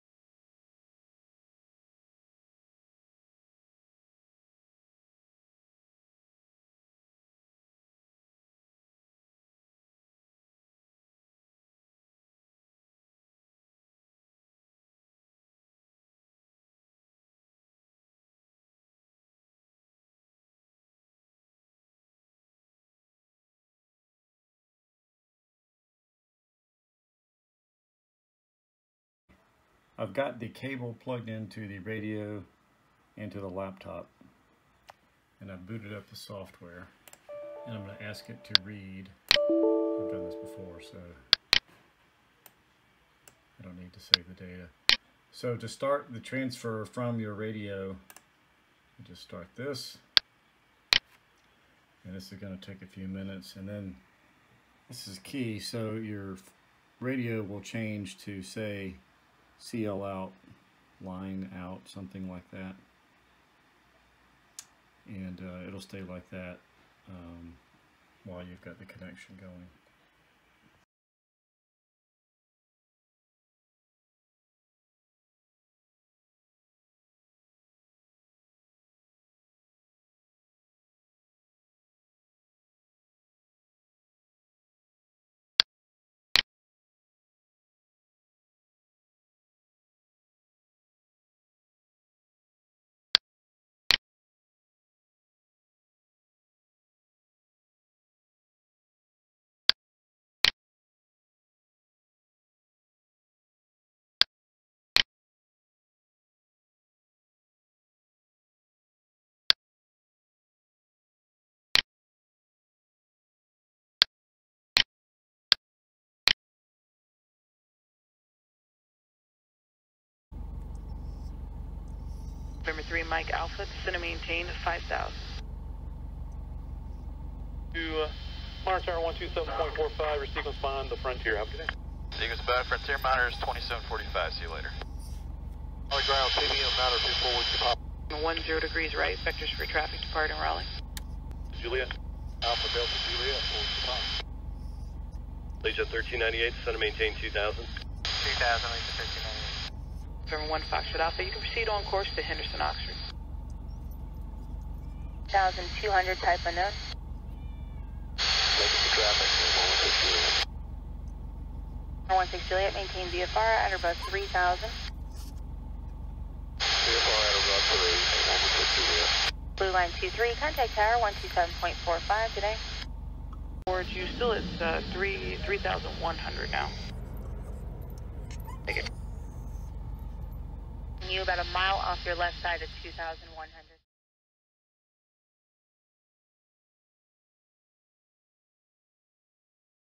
Programming an ICOM IC-R6 wideband radio receiver
Video showing how to program an IC-R6 radio using ARC6 software and a USB-to-serial cable. In this case I programmed specific frequencies of two airports into two memory banks. Then used the scan feature to listen.